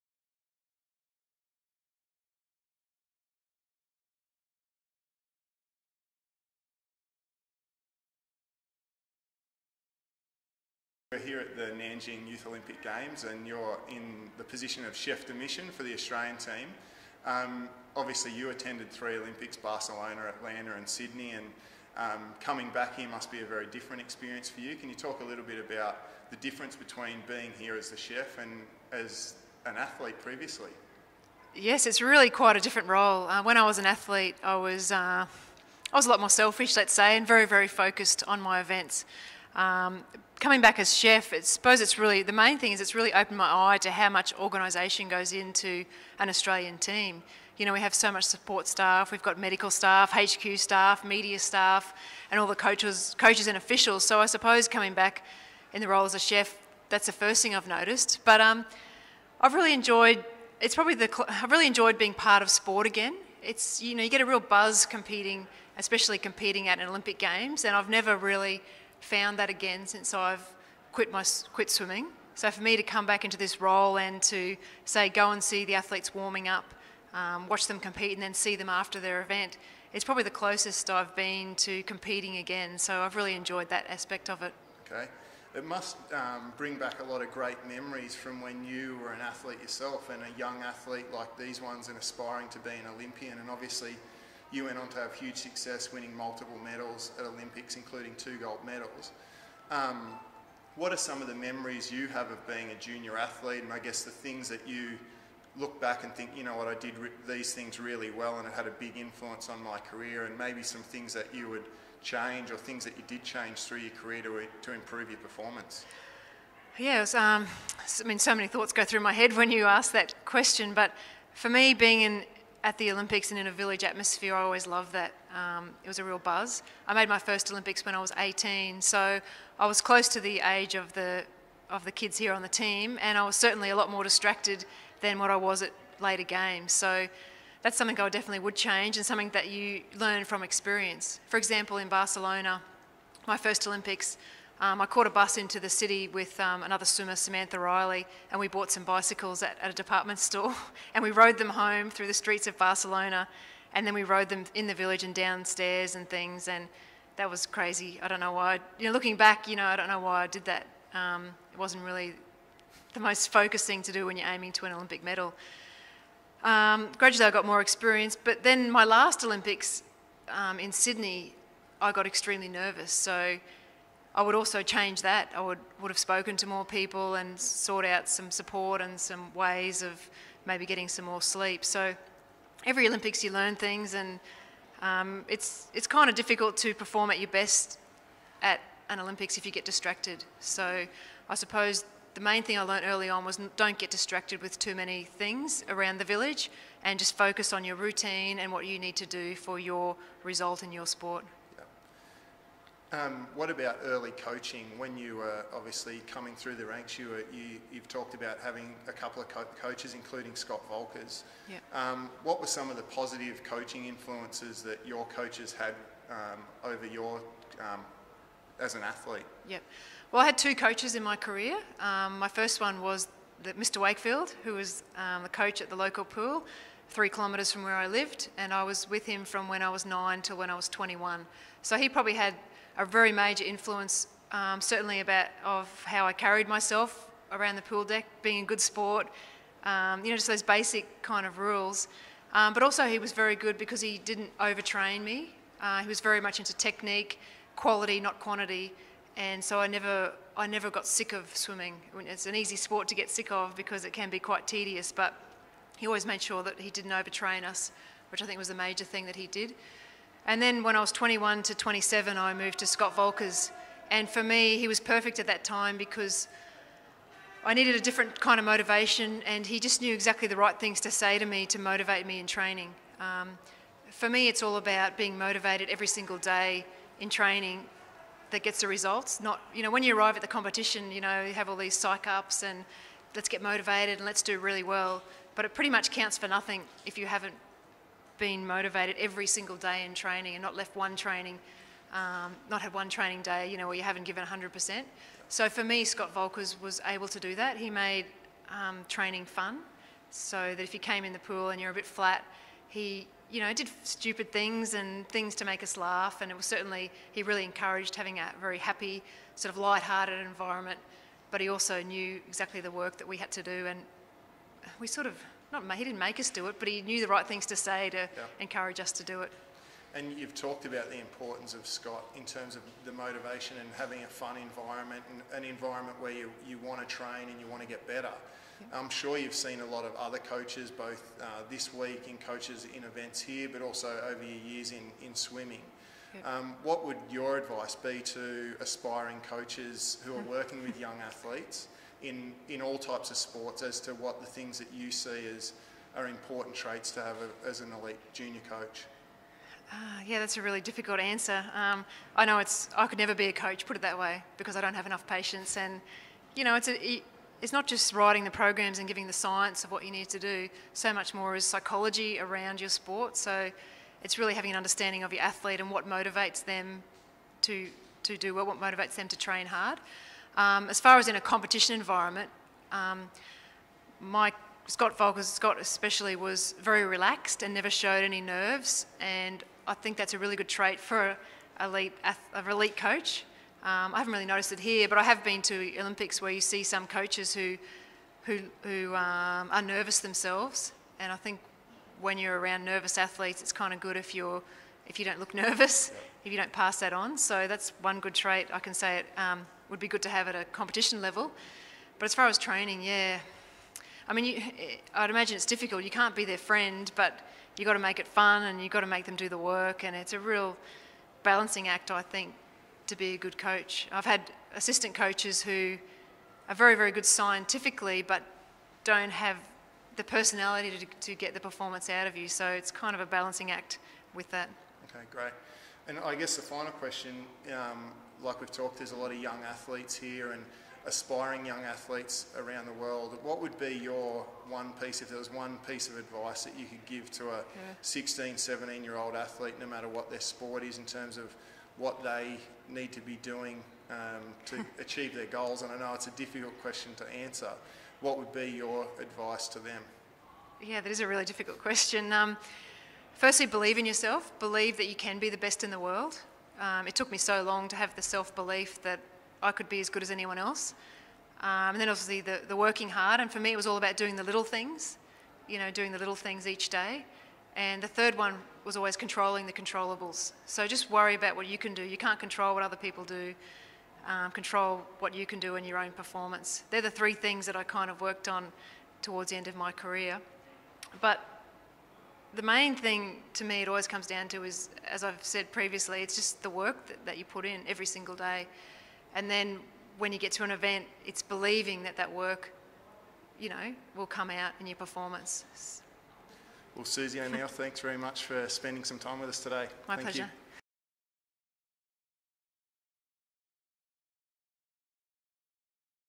Susie O'Neill talks about becoming a successful athlete.
One of Australia’s most successful ever swimmers, Susie O’Neill holds a record 35 Australian titles, 8 Olympic medals and a string of victories at the World Championships and Commonwealth Games. After narrowly missing selection as a fourteen year old for the 1988 Seoul Olympics, Susie’s career took off at the 1990 Commonwealth games with gold and silver medals. Incredibly, Susie never came home without a medal from any international competition, a streak stretching from 1990 to 2000. Susie came of age at the 1996 Atlanta Olympics, a gold, silver and bronze medal; she was Australia’s most outstanding Olympic performer since Shane Gould in 1972 and the first Australian female swimming Olympic gold medalist since 1980.

At the recent Youth Olympic Games in Nanjing China, Susie spoke with Tony Meyer about her career, what her greatest influences were and what it takes to be a successful athlete.